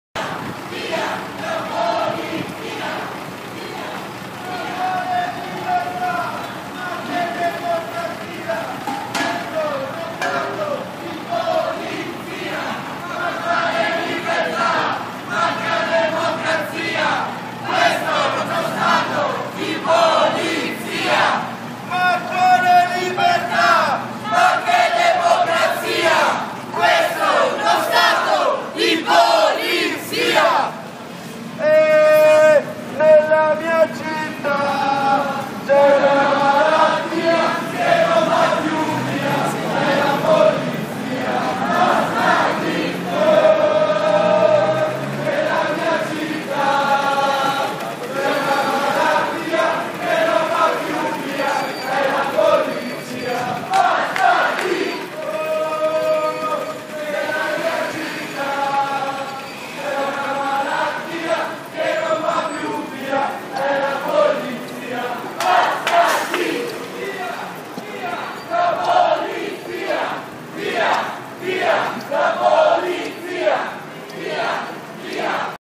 Manifestazione del C.S. Pedro a Padova
Protesta questo pomeriggio davanti alla Questura di Padova  dei giovani appartenenti al Centro Sociale Pedro. La prot... [continua a leggere su